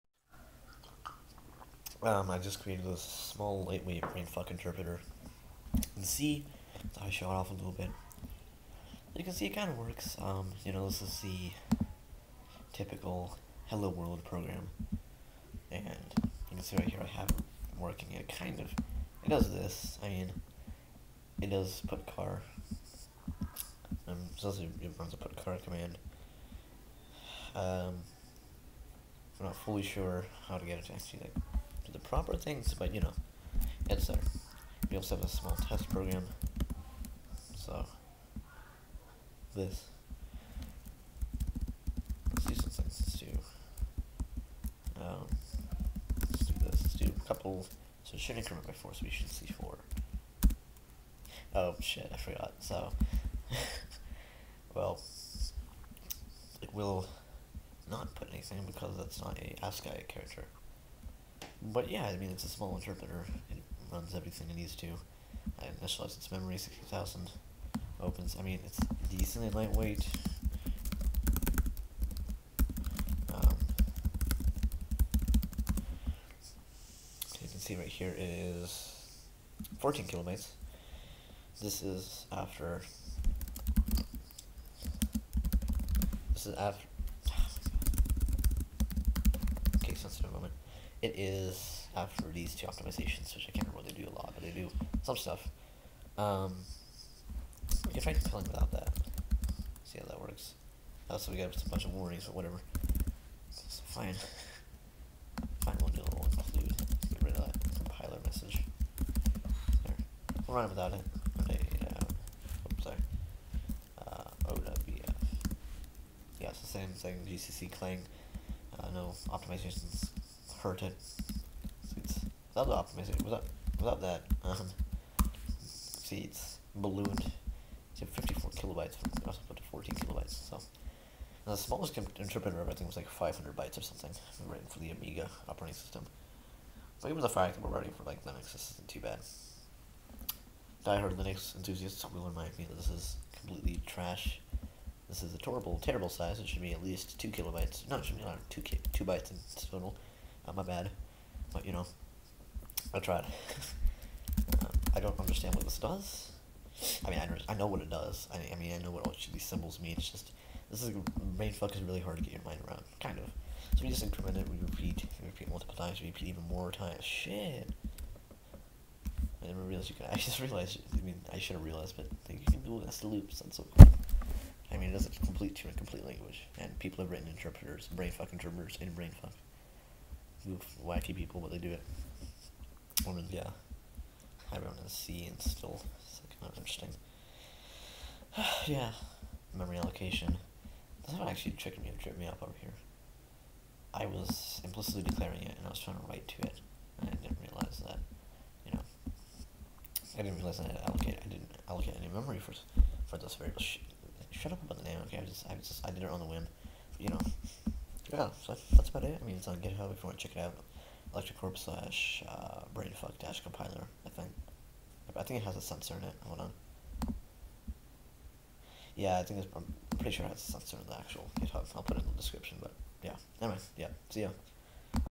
A demo of my small BrainF*ck interpreter.
I wrote a small BrainF*ck interpreter in C. Not perfect, but it works.
[Sorry about the mic, I'm not sure what went wrong. Weird white noise in some places]